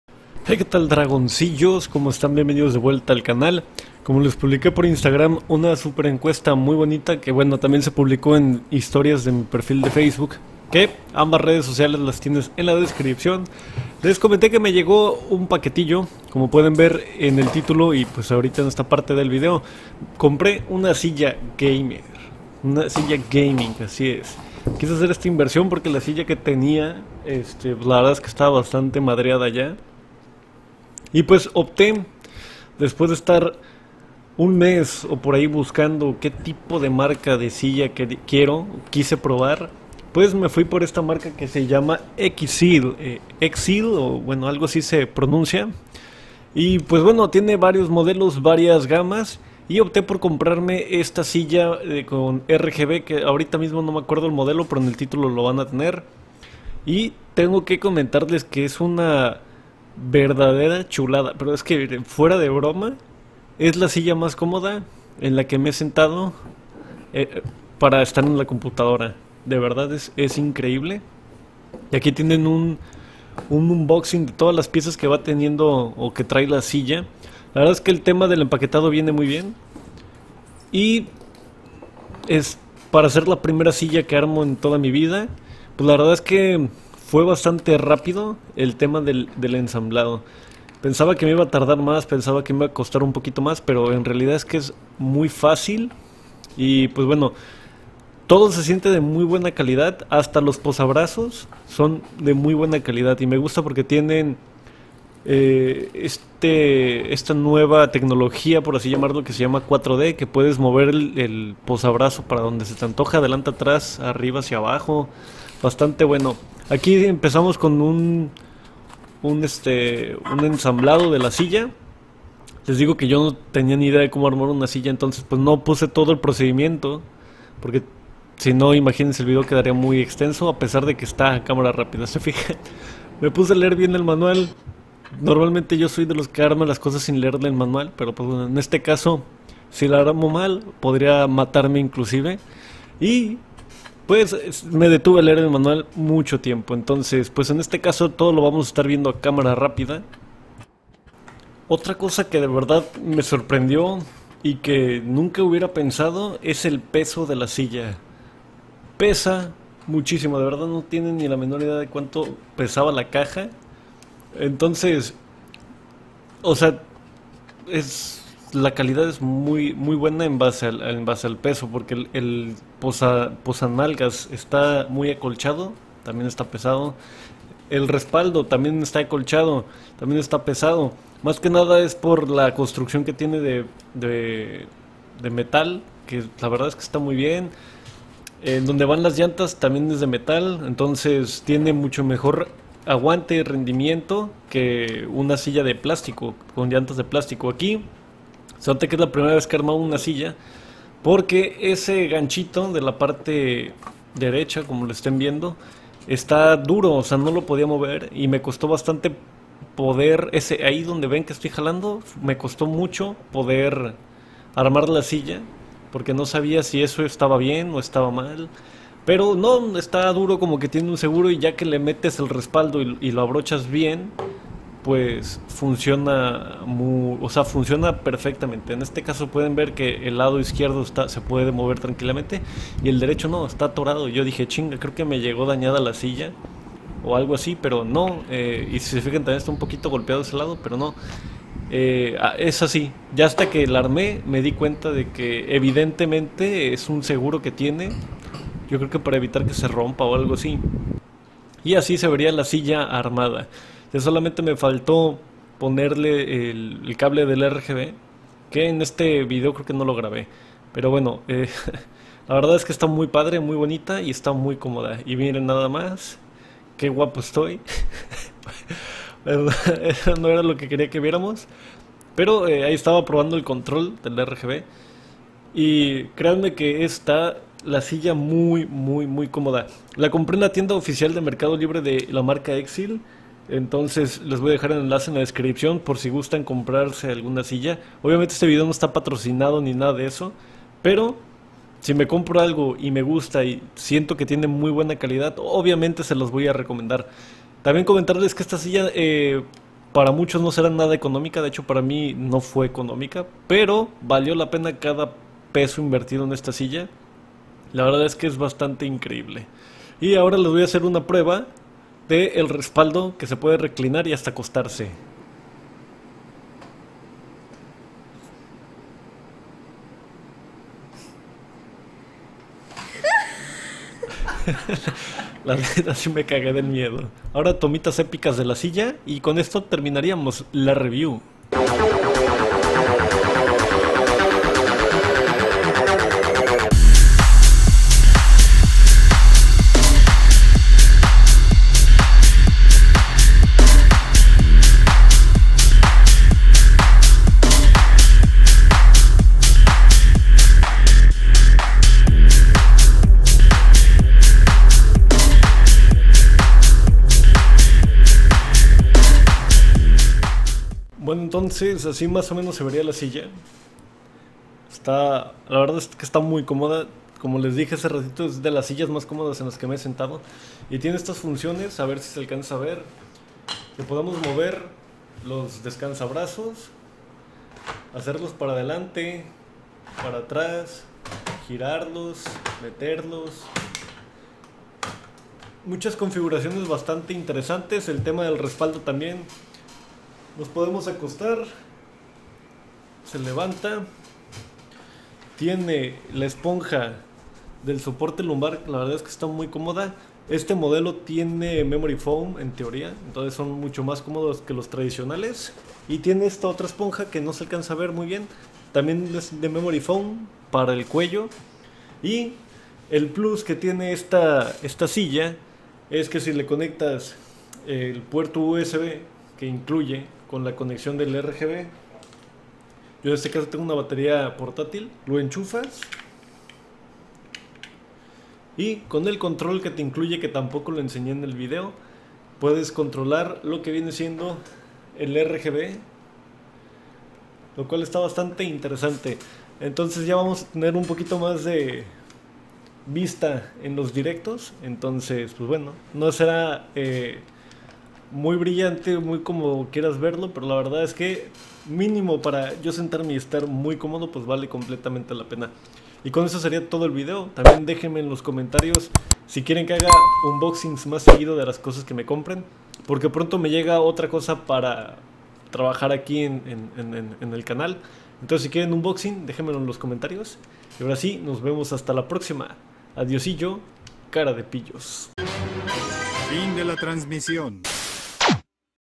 Unboxing XZEAL XZ15 RGB 😍 | Me compre una silla GAMER con Luces para mas poder
Y obtén múltiples beneficios